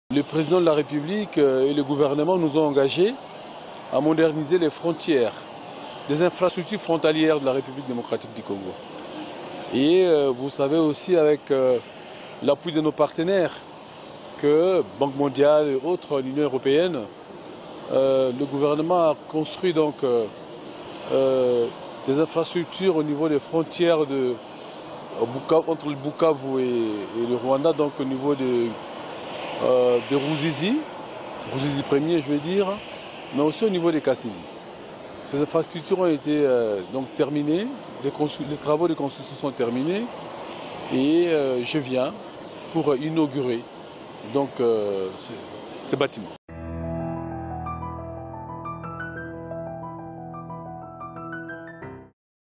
RDC/Nord Kivu : Jean Lucien Busa à Goma pour l’inauguration des infrastructures frontalières
Le ministre du Commerce extérieur, Jean-Lucien Busa Tongba, est arrivé à Goma dans la mi-journée de ce mardi 23 août 2022, dans le cadre d’une mission d’inauguration des infrastructures frontalières à Kasindi, dans le territoire de Beni au Nord-Kivu ainsi qu’à Ruzizi, dans la province du Sud-Kivu.